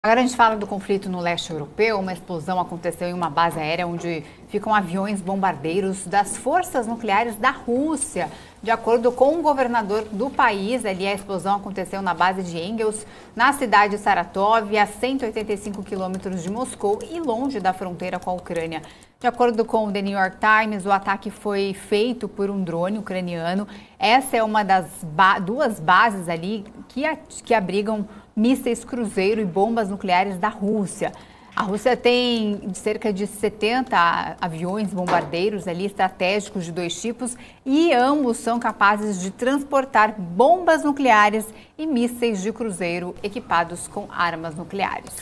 GUERRA NO LESTE EUROPEU: BASE AÉREA NUCLEAR RUSSA É ATINGIDA POR EXPLOSÃO
GUERRA NO LESTE EUROPEU: Base área onde ficam aviões das forças nucleares da Rússia foi bombardeada nesta segunda-feira.
Aeródromo na cidade de Kursk foi incendiado depois de um ataque nesta terça-feira, 6.
Uma terceira base militar russa foi incendiada por um ataque de drone. Outras duas bases no interior da Rússia haviam sido atacadas, segundo as autoridades Russas, pela Ucrânia, o que demonstrou uma aparente nova capacidade ucraniana de penetrar centenas de quilômetros no espaço aéreo russo.